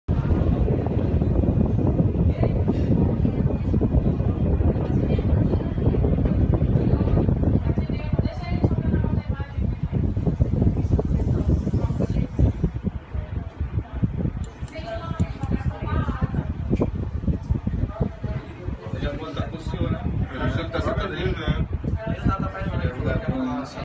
Dupla invade autoescola na Zona Sul e rouba funcionários
Câmeras do circuito interno de segurança da autoescola registraram a ação criminosa da dupla – foto: reprodução